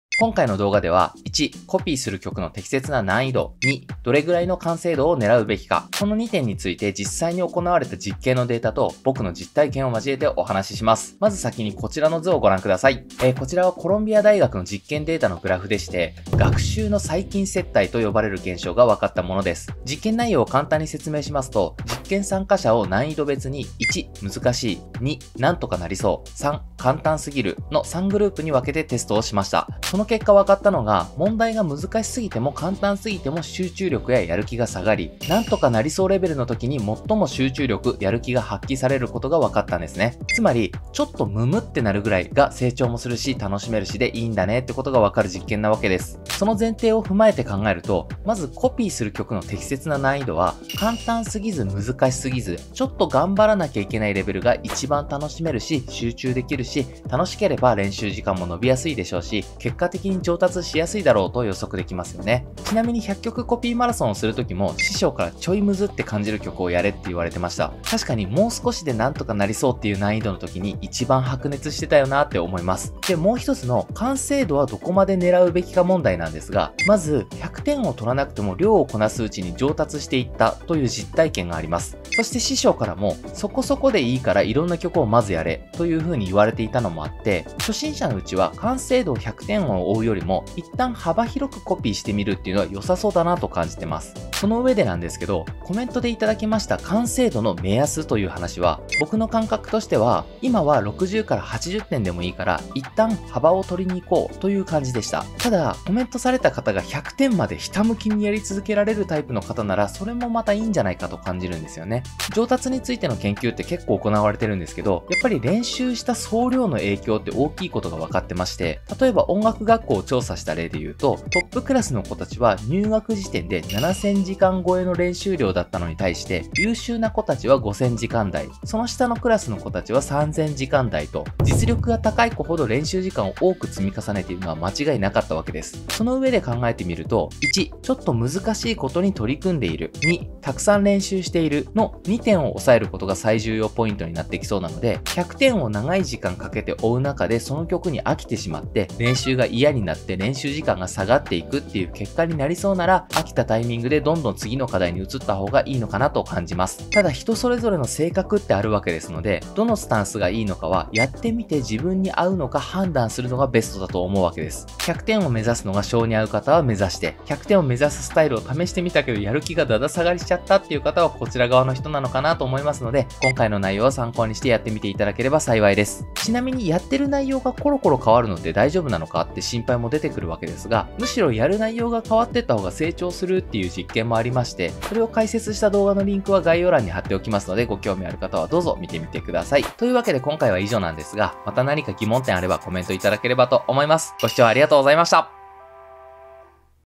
ドラム歴2年が1年で100曲コピーした時の難易度と完成度について
頑張りたいけどやる気が出ない…と悩むのはよくある話なんですが、「そもそもやる気が出ないのは難易度設定が間違えているのでは？」ということをコロンビア大学が研究してくれてまして、この話が楽器練習での「コピー曲の難易度設定」にも使えるな～ということで解説させて頂きました。
また合わせて、以前コメントで頂きました「1曲1曲の完成度はどこまで狙うか問題」についても話してみましたので、参考にして頂ければ幸いです。

動画内でご紹介した「ランダムな練習の方が76％も上達しやすい研究」の動画はこちら

画像を引用させて頂いたコロンビア大学のページはこちらです(英語です)